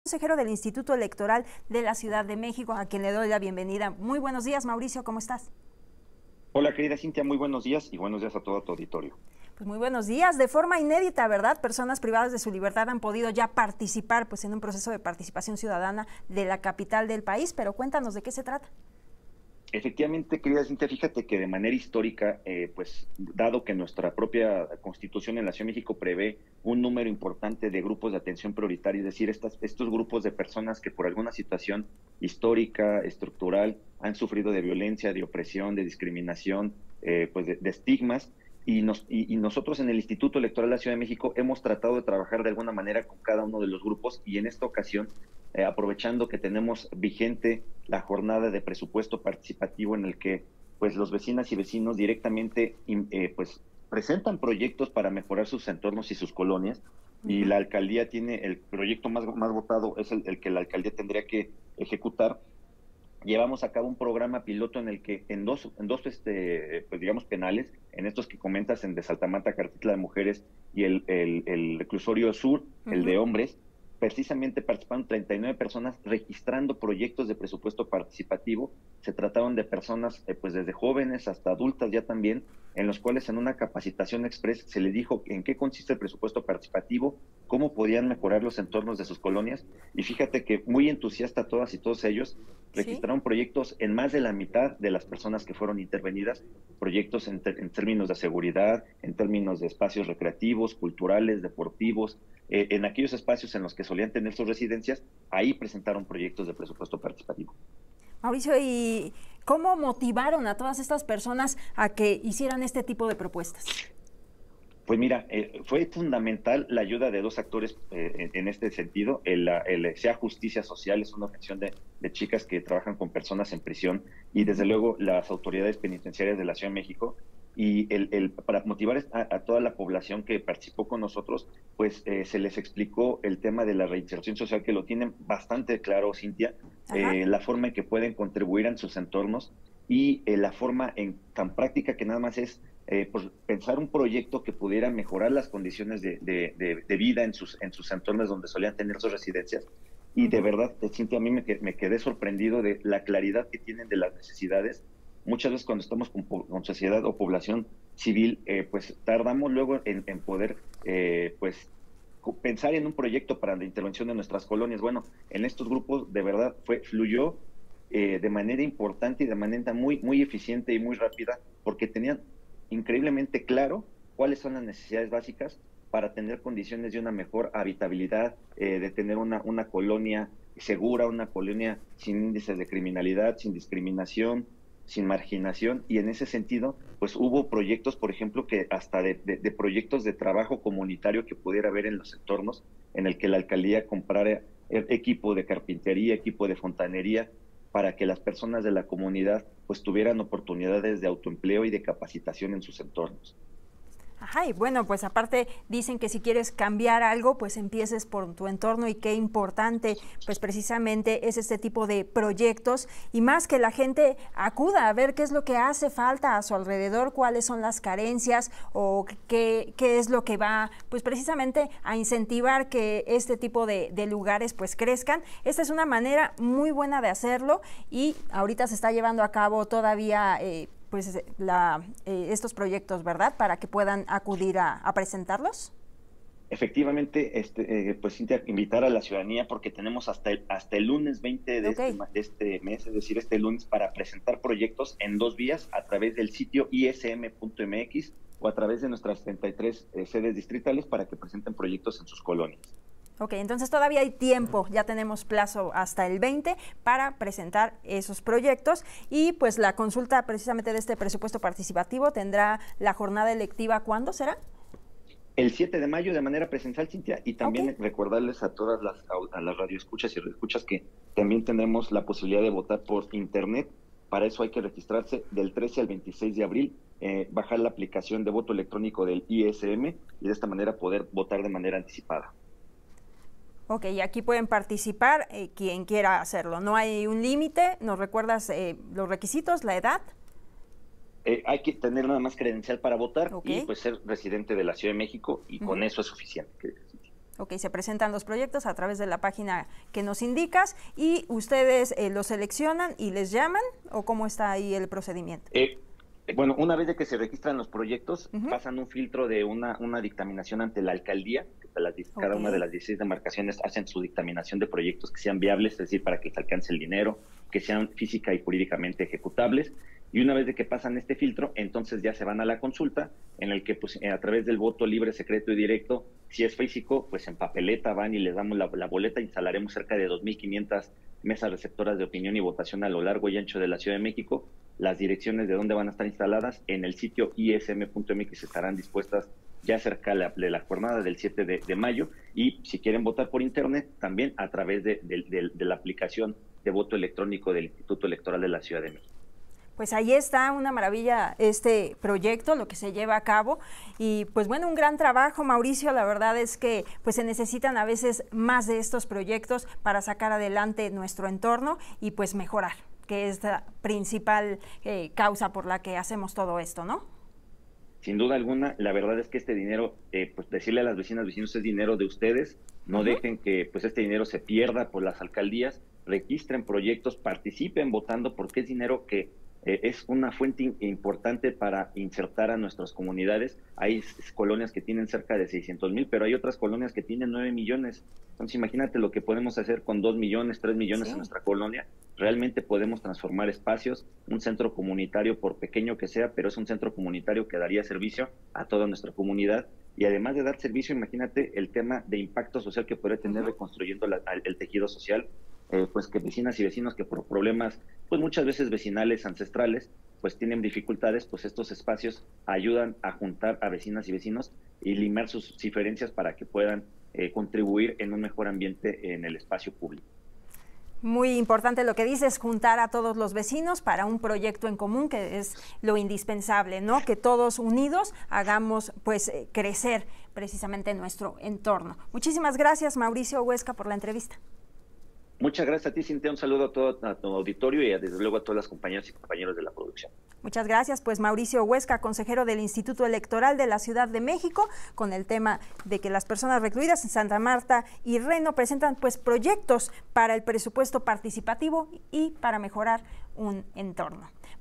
¡Histórico! Personas privadas de la libertad colaboran en Presupuesto Participativo en CDMX
En una decisión histórica, por primera vez en la CDMX personas privadas de la libertad colaboran en Presupuesto Participativo. Esta iniciativa permite que ciudadanos propongan proyectos en favor de sus colonias para atender algún problema social.